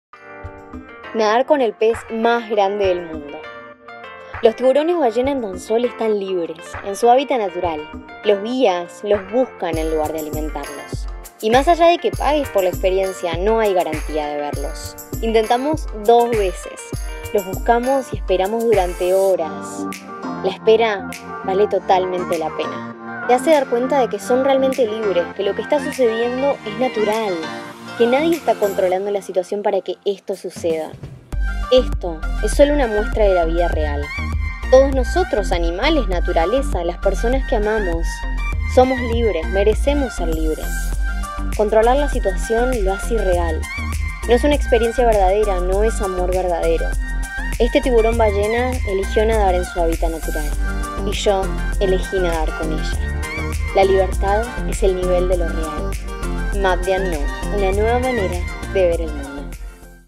Swimming With The Biggest Fish In The World
True or fake experiences? Swimming with this big whale shark was more than that, it show us the difference of the feeling when something is natural than when something is forced and controlled.
This not only happen on the excursions with animals, it happens also in your day by day, in your relationships. Freedom is the level of real. Watch this video, we hope this makes you reflect on which type of experiences are you looking for in your life.
¿Experiencias verdaderas o falsas? Nadar con este gran tiburón ballena fue más que eso, nos mostró la diferencia entre el sentimiento cuando algo es natural y cuando algo es forzado o controlado.
Esto no solo sucede en las interacciones con los animales, sino que también ocurre en nuestras relaciones. La libertad es el nivel de lo real. Mirá este video, esperamos que esto te haga reflexionar sobre qué tipo de experiencias y relaciones busca en tu vida.